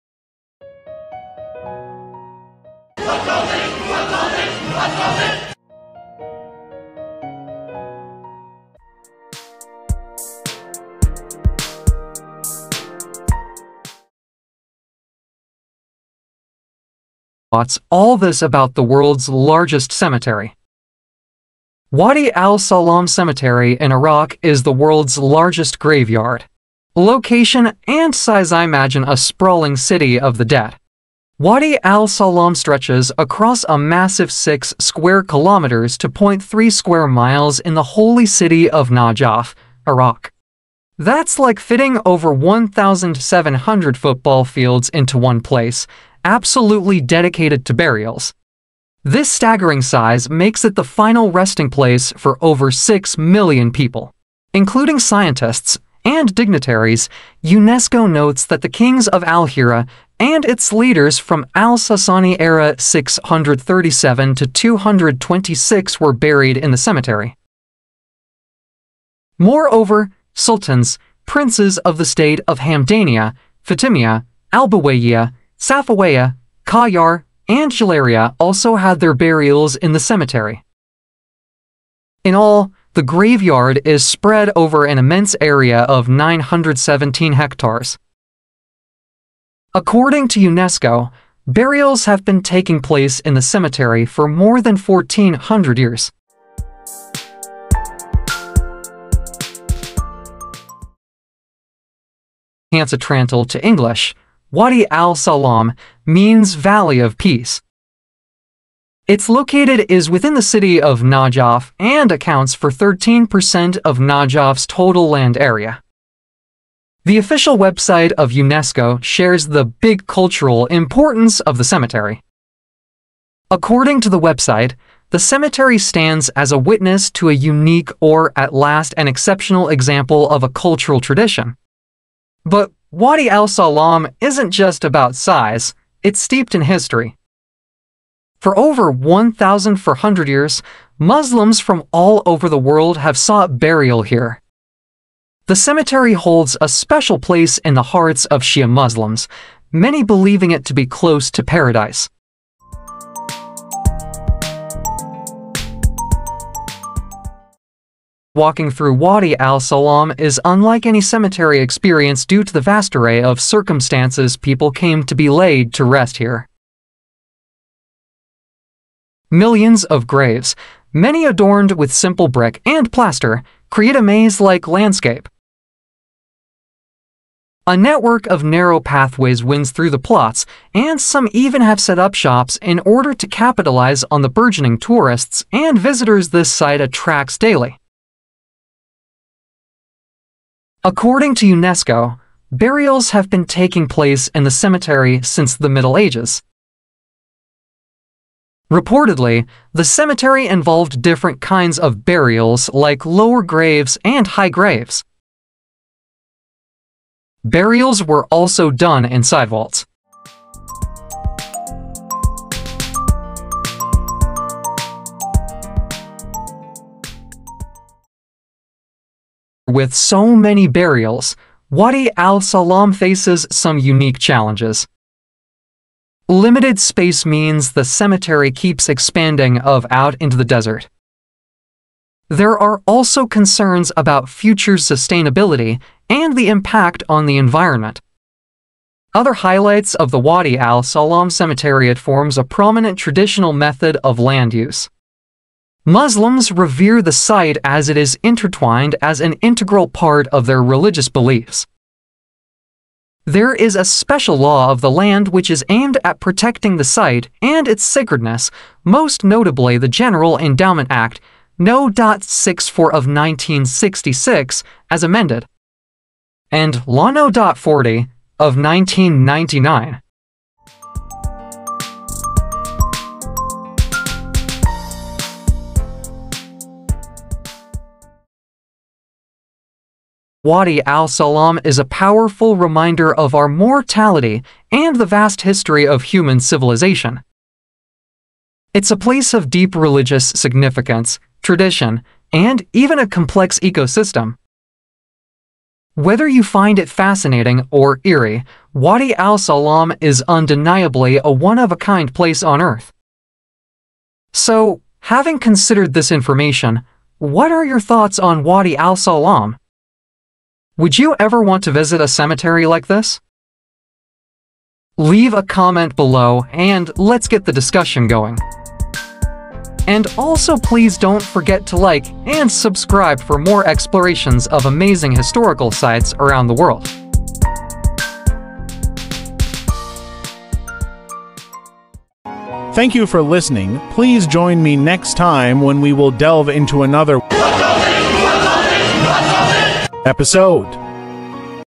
Visiting Wadi Al-Salam in Iraq: A Look Inside the Largest Graveyard on Earth
Forget everything you think you know about cemeteries!  We're journeying to Wadi Al-Salam, a mind-blowing 6-square-kilometer graveyard in Iraq – the BIGGEST on Earth!  This labyrinth of millions of graves holds mysteries, historical significance, and even a few chills.  Is it a holy site or an eerie expanse?  We'll explore the unique customs, uncover the stories beneath the simple brick headstones, and discuss the future of this ever-growing "City of the Dead."  Buckle up for a fascinating (and maybe a little spooky) adventure in Wadi Al-Salam!

This MASSIVE 6km² necropolis in Najaf, Iraq, holds the remains of MILLIONS over 1400 YEARS.  Explore the labyrinthine layout, uncover its significance for SHIA MUSLIMS, and delve into the CHALLENGES of this ever-expanding CITY OF THE DEAD.  Is it a haunting reminder of mortality or a sacred pilgrimage site?  Join us for a DOCUMENTARY-STYLE exploration of WADI AL-SALAM!